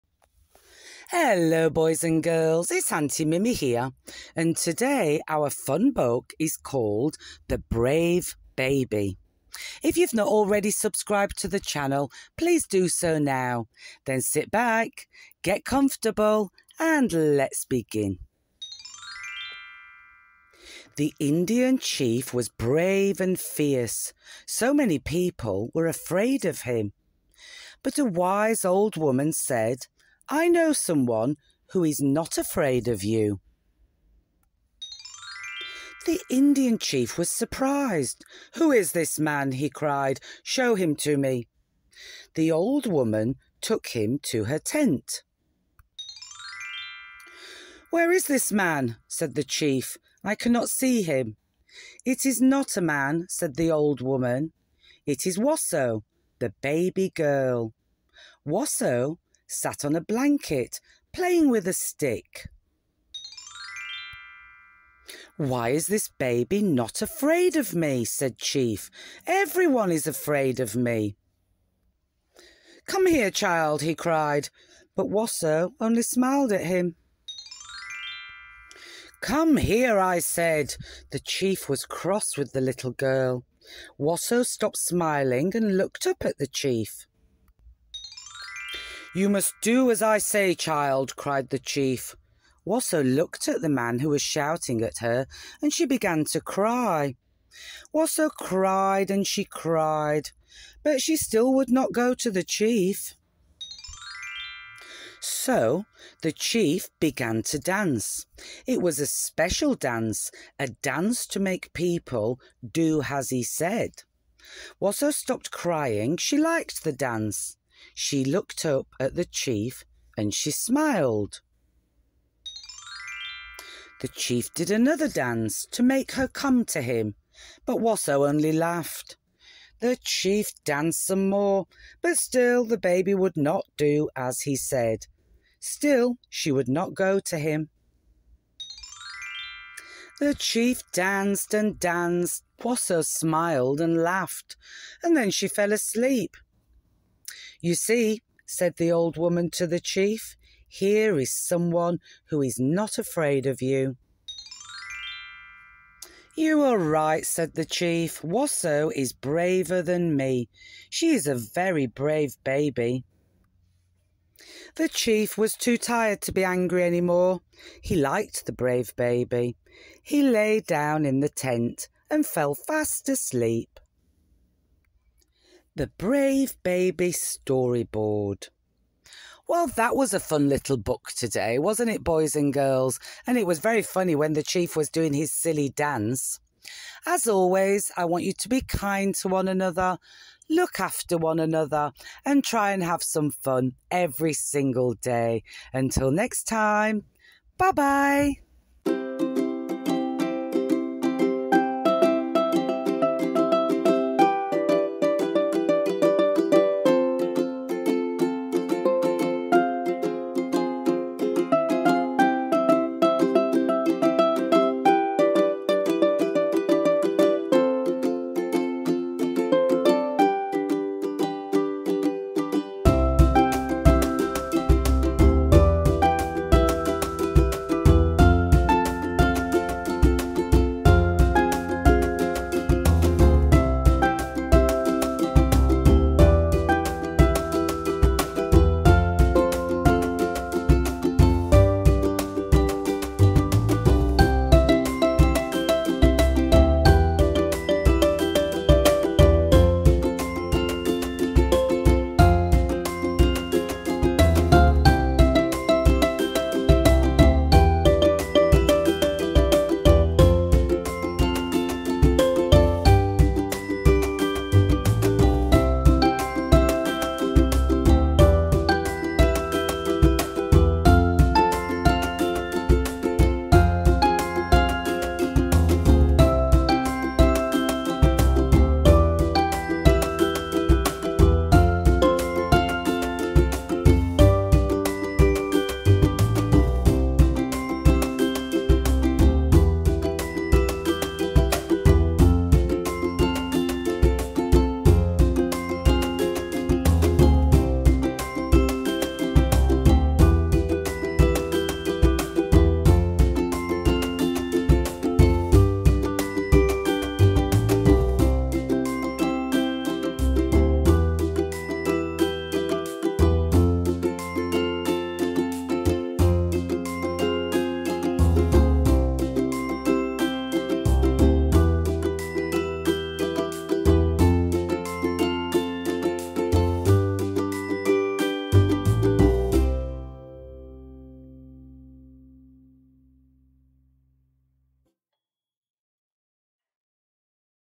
THE BRAVE BABY, Read By Aunty Mimmi, Fun Books For Children...
The Indian Chief was brave and fierce!
 But Wasso, the brave baby was not afraid of him!
A fun little book for young children.